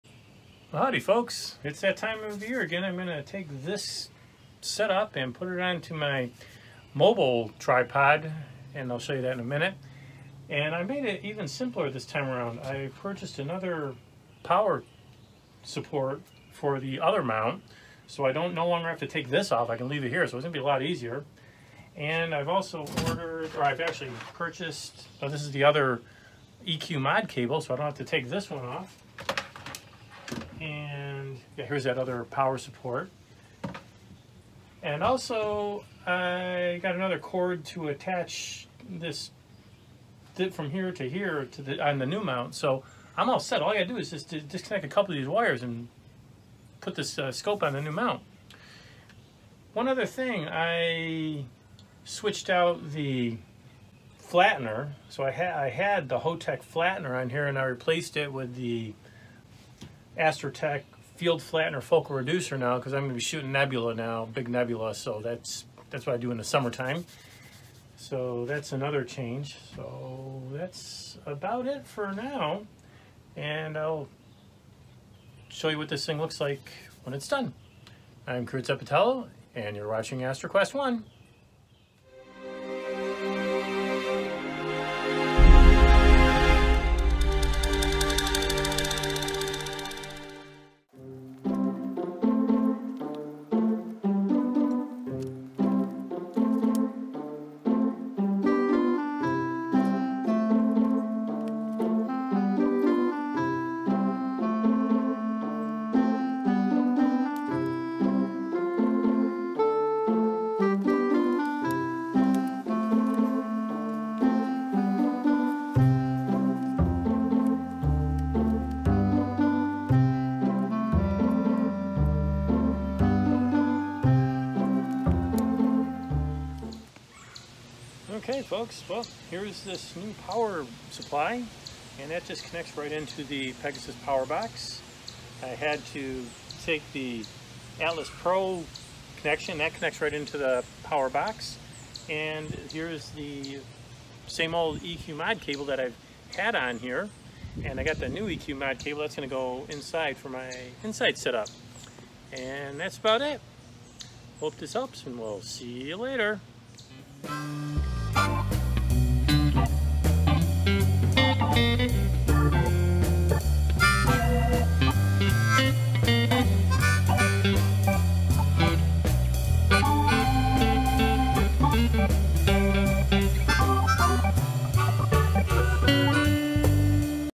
Switching from the Winter Mount (Sirius) to the Summer Mount (Atlas Pro) 2020
I added a couple of power supply and cables as well as a new ZWO 60mm Guidescope (not shown on the video).  Double Feature: I made another video the same day on the Scopebuggy Upgrades as well.  That video is coming soon.